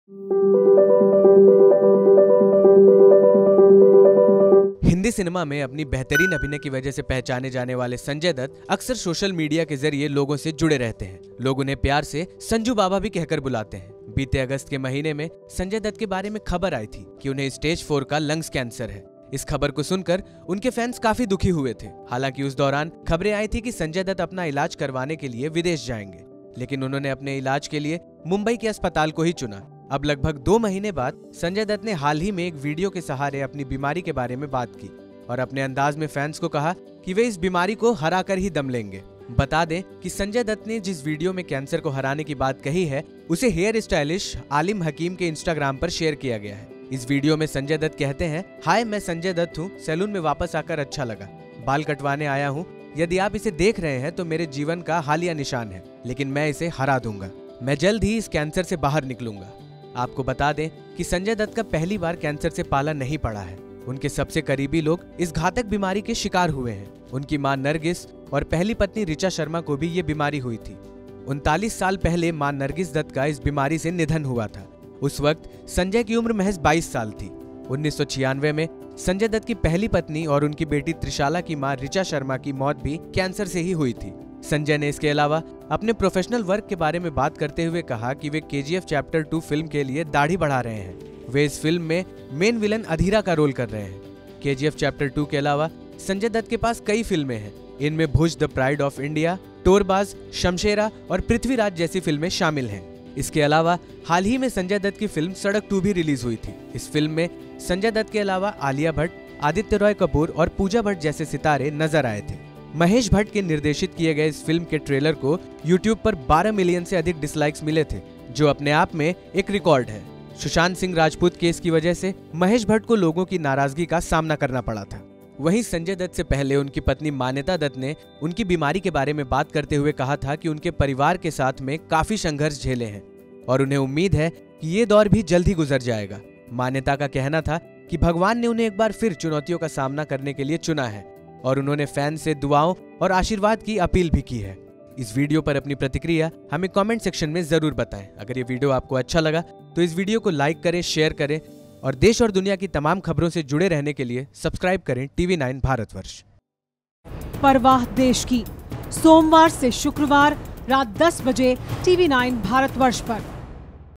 Cancer की बीमारी से जूझ रहे Sanjay Dutt का नया वीडियो आया सामने
हिन्दी सिनेमा में अपने बेहतरीन अभिनय की वजह से पहचाने जाने वाले संजय दत्त (Sanjay Dutt) अक्सर सोशल मीडिया (Social media) के जरिए लोगों से जुड़े रहते हैं. लोग उन्हें प्यार से संजू बाबा भी कह कर बुलाते हैं. बीते अगस्त के महीने में संजय दत्त के बारे में खबर आई थी कि कि उन्हें लंग्स कैंसर (Lung cancer) है. इस खबर को सुनकर उनके फैंस काफी दुखी हुए थे.